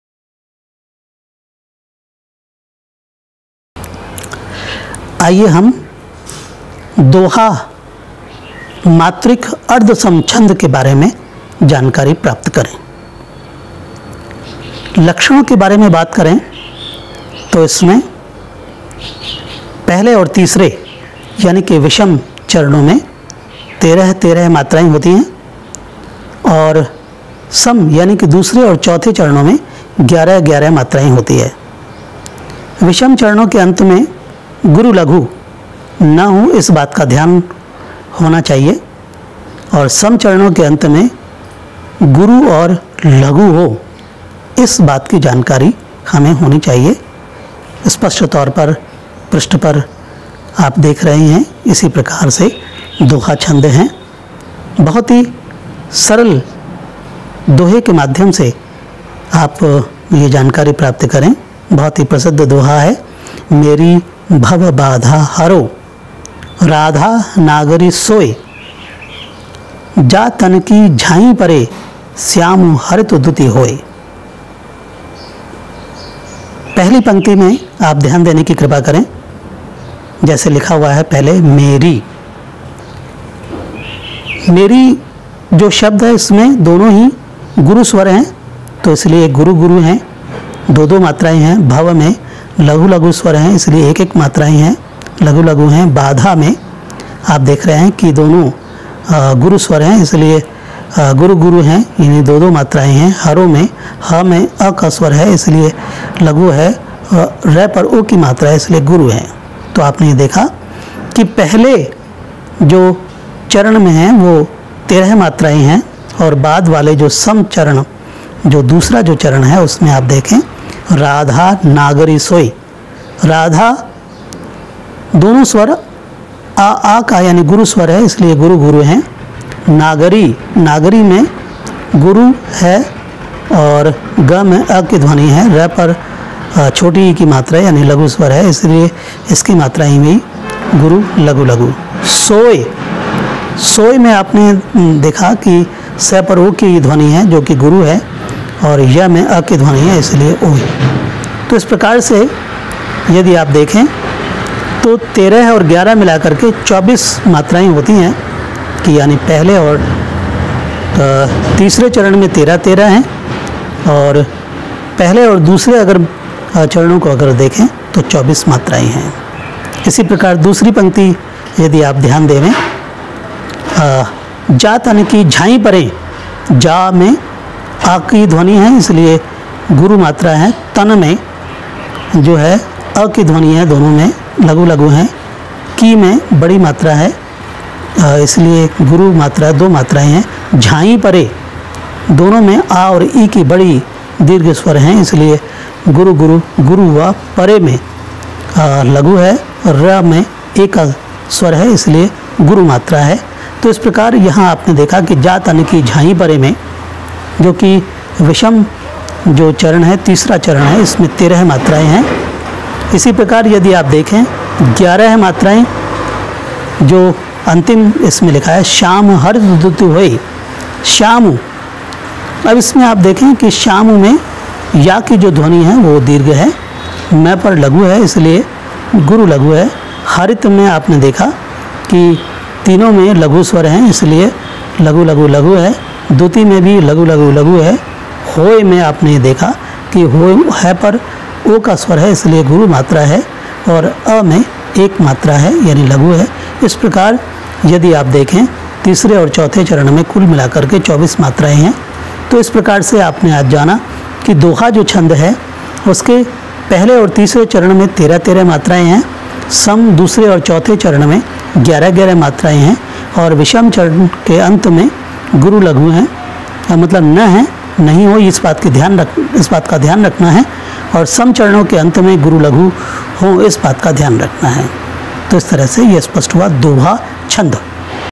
हिन्दी साहित्य के विद्यार्थियों के लिए ' दोहा छंद ' का परिचय...डॉ.अनिल शर्मा..Dr.Anil Sharma...
डॉ.अनिल शर्मा..Dr.Anil Sharma...